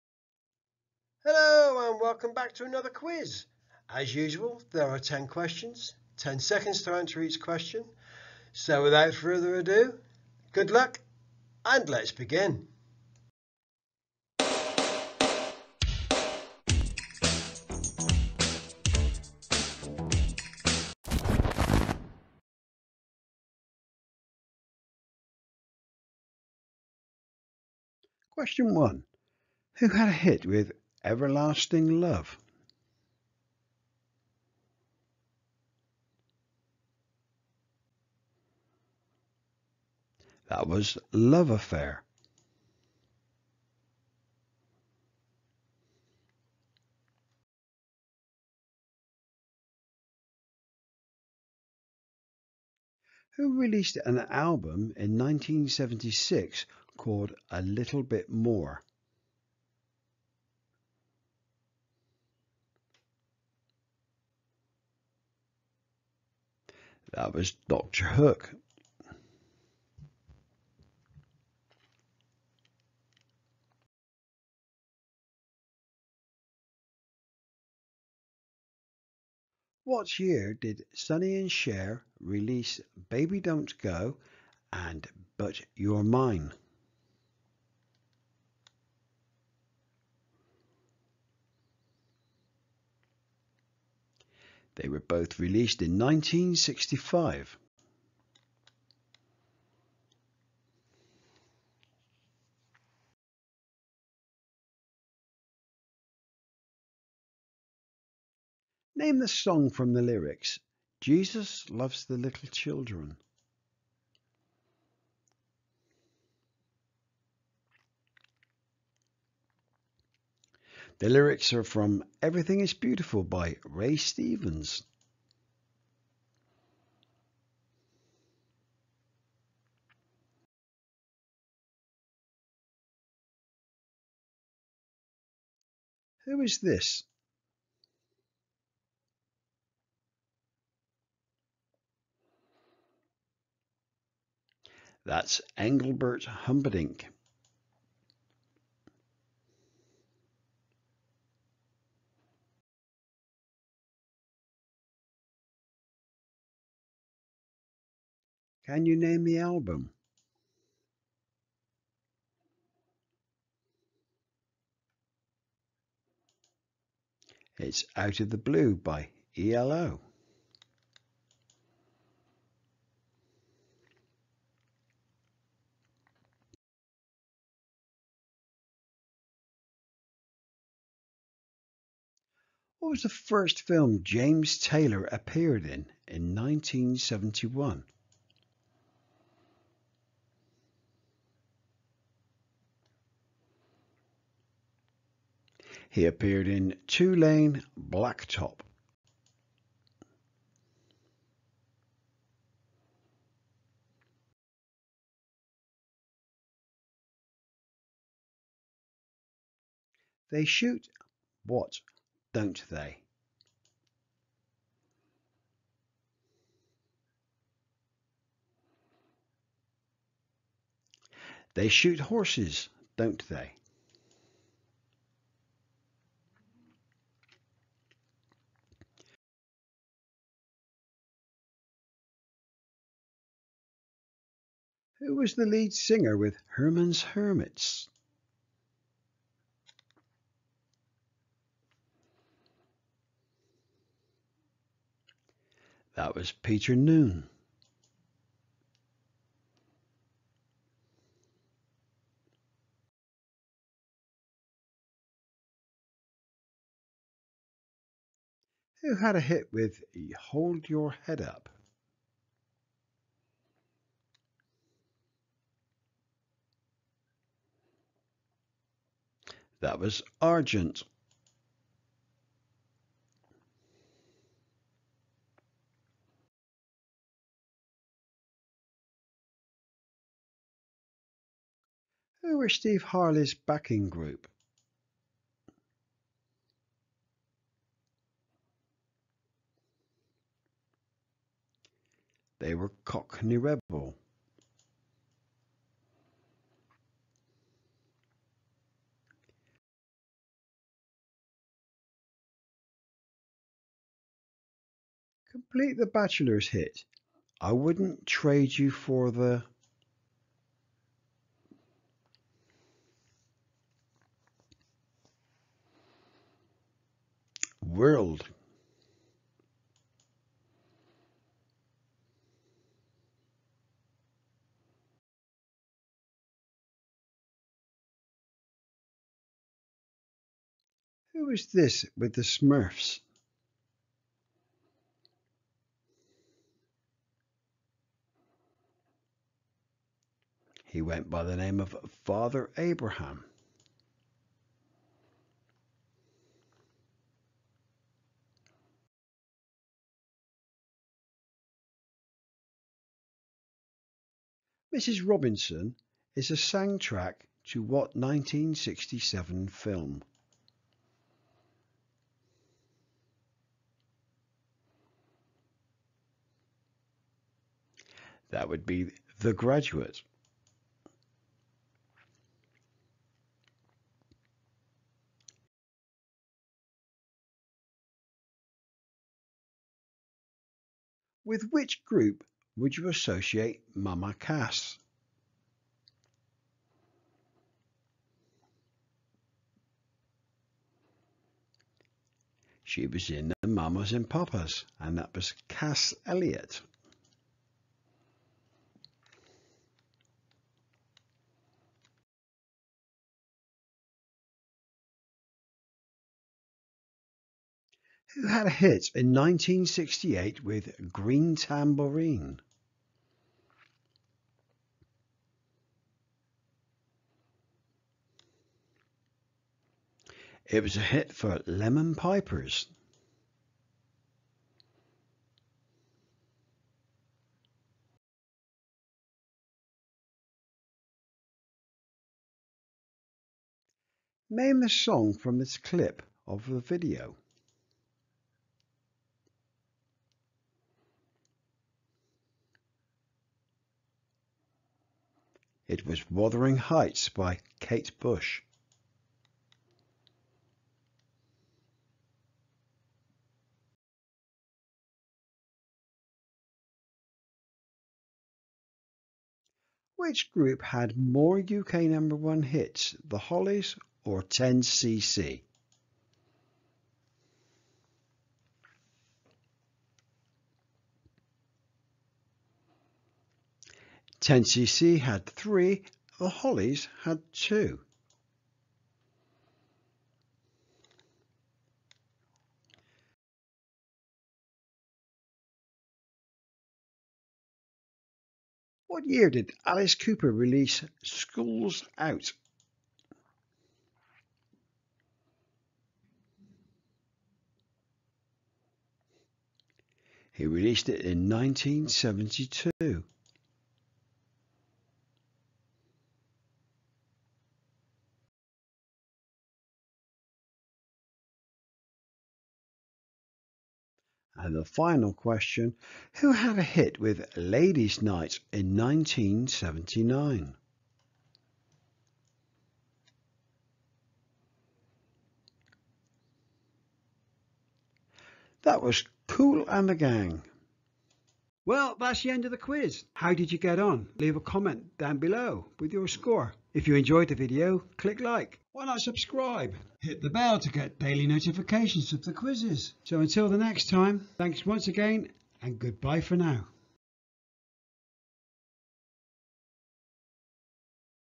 Pop Music quiz 1960s & 70's No 23
Can you name the hit, the band, the album from groups the 60's/70's?
How about a manager or a record label or a guest appearance?
If you enjoy quizzes then you've come to the right place.
How good is your memory of 60's and 70's music?
Test yourself with this music quiz.
I've tried to keep the quiz interesting with dates and such, so even if you don't get them all right hopefully you'll find out something you didn't know about the ones you missed.
Good luck